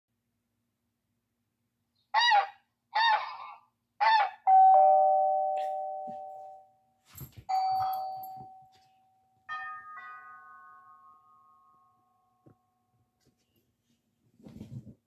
Adorable Shih Tzu/Maltese puppy reacting to different sounds
Bruno reacts to different sounds.